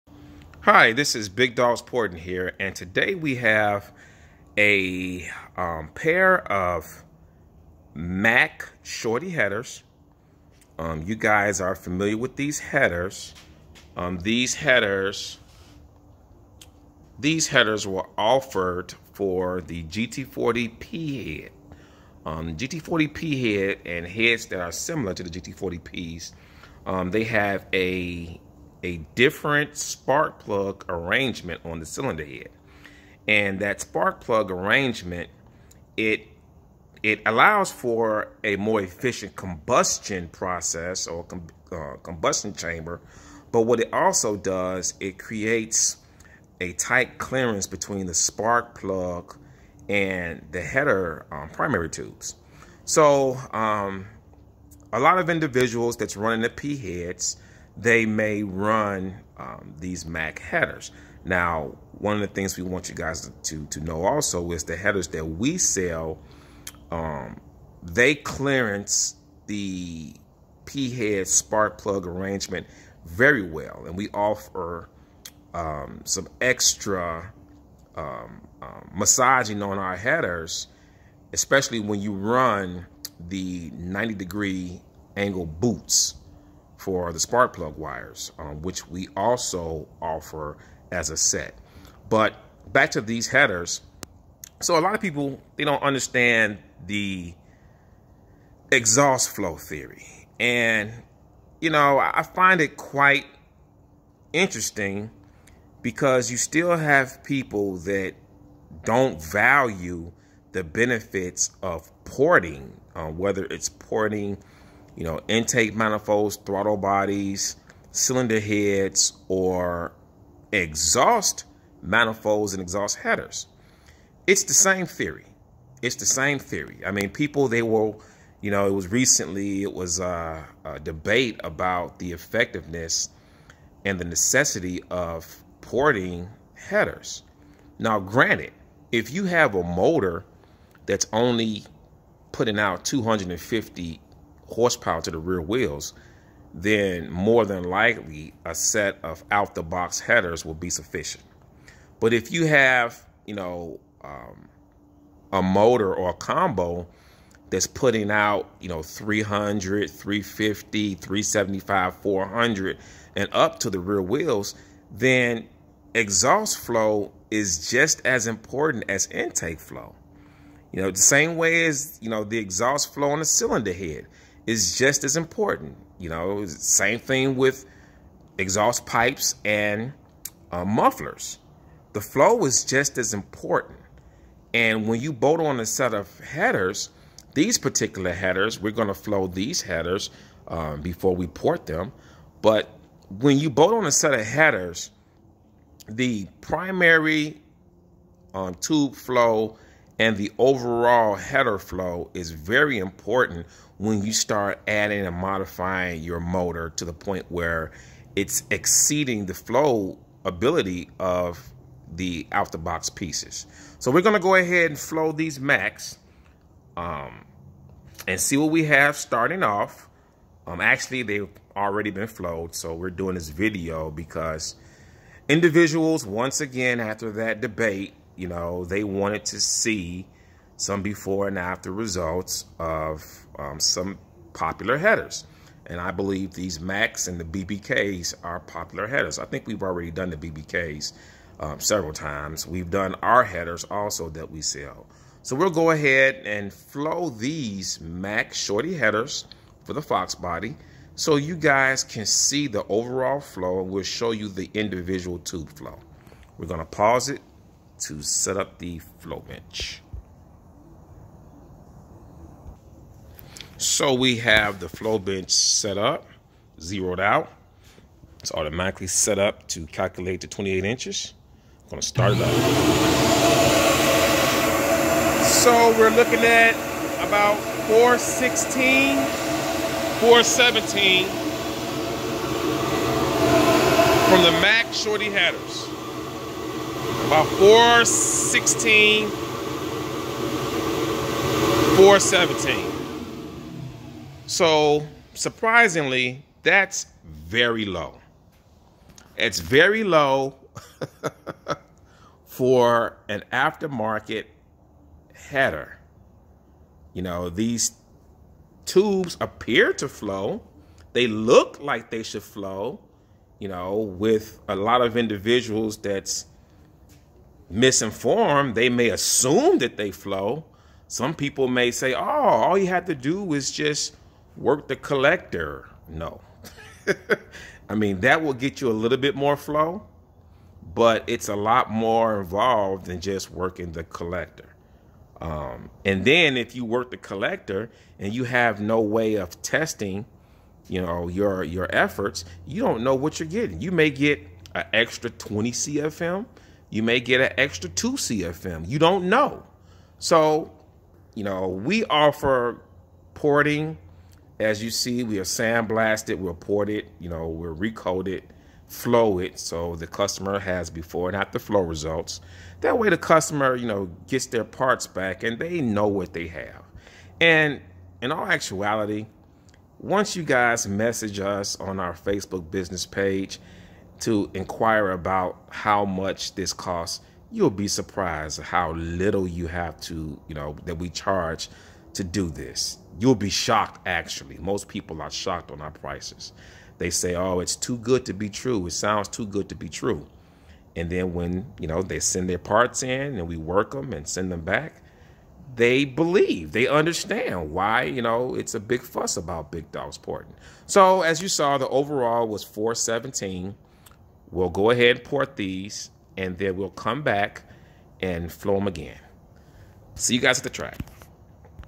Fox Body Mustang Mac shorty Header Flow testing part 1
As requested by a numerous number of customers that wanted to see REAL WORLD before and after flow results on the Mac headers here's the part 1 before video. These are customers headers that were sent in to receive our ported, flow testing, welding for strength, sand blasting and 2,000 degree coating all for a dirt cheap price.